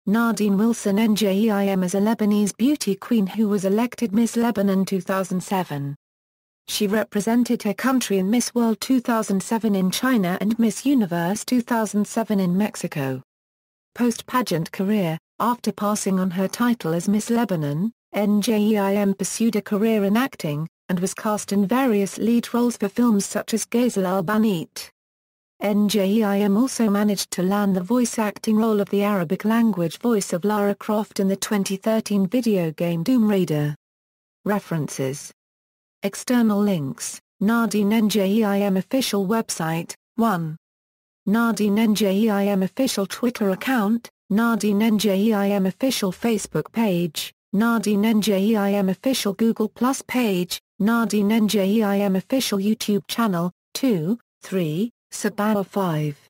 Nadine Njeim
Nadine Wilson Njeim is a Lebanese beauty queen who was elected Miss Lebanon 2007. She represented her country in Miss World 2007 in China and Miss Universe 2007 in Mexico.